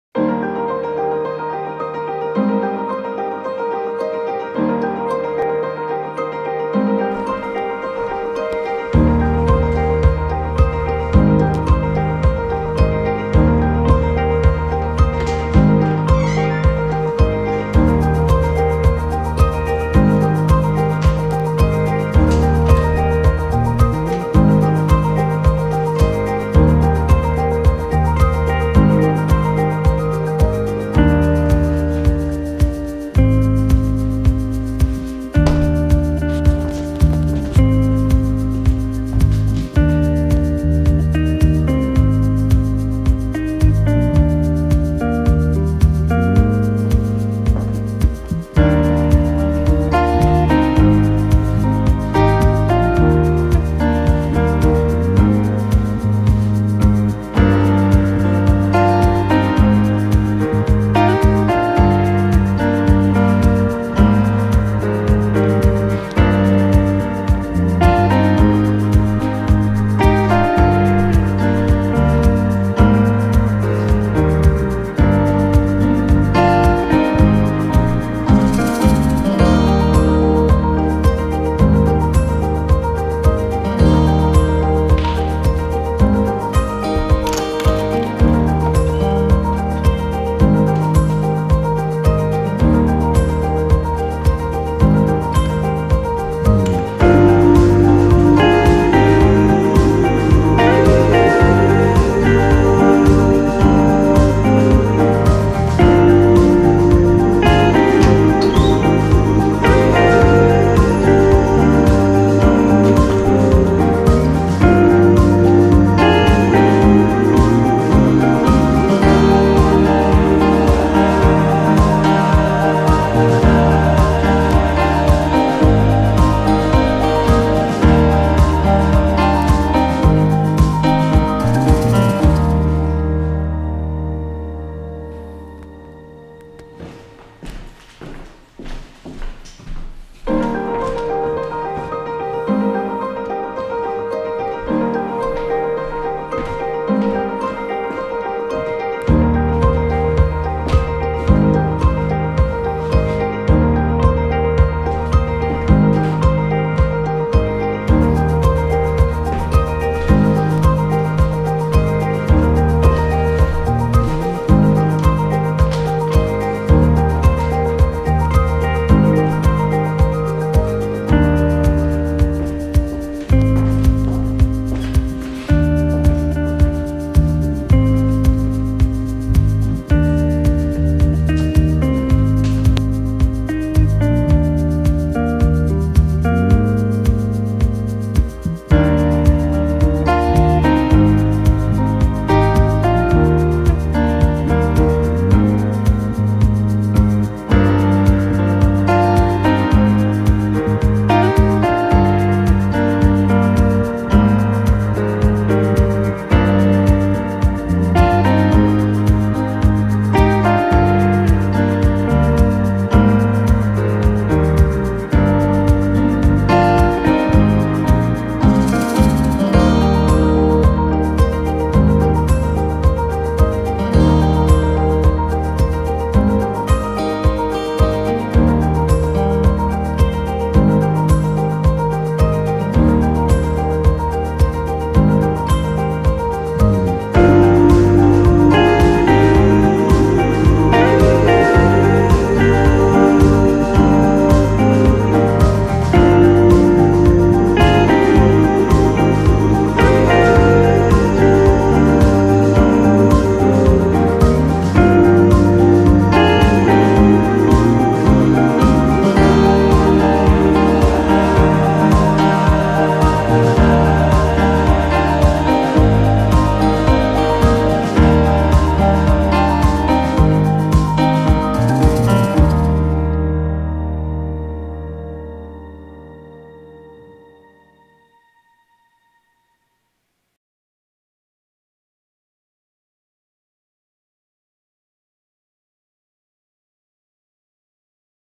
Prospect Heights 2br U2apartment tour $2,200
Prospect Heights 2br Under Market tour
Heat & hot water included
near Prospect Park
Near trains